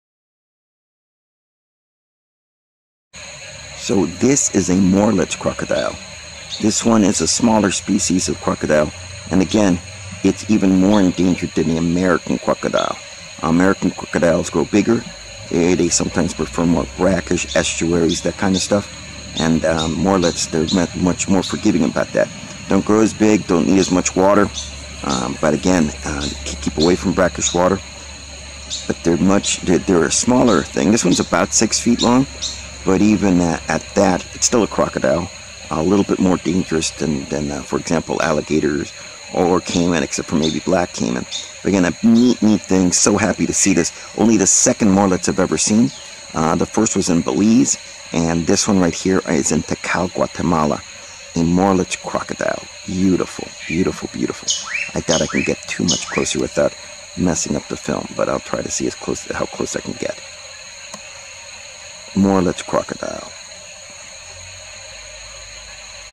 Capital Naturalist: Morelet's Crocodile
Morelet's Crocodile (Crocodylus moreletii) are also known as mexican Crocodiles or Belize Crocodiles, are smaller than American Crocodile and have a more restricted range.